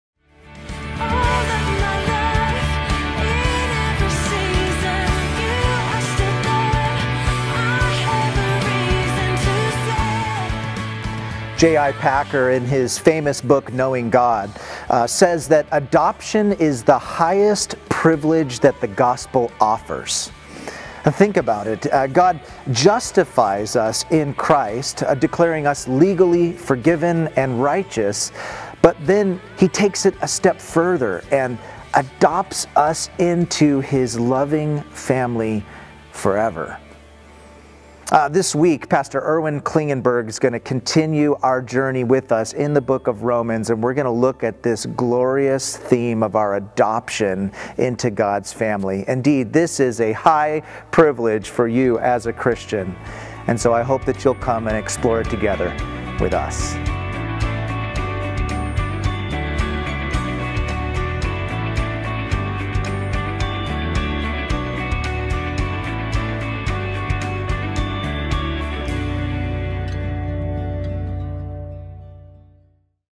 Our Identity in the Spirit - Preview
A preview of the sermon this Sunday, February 23, 2020.
Text is from Romans 8:12-17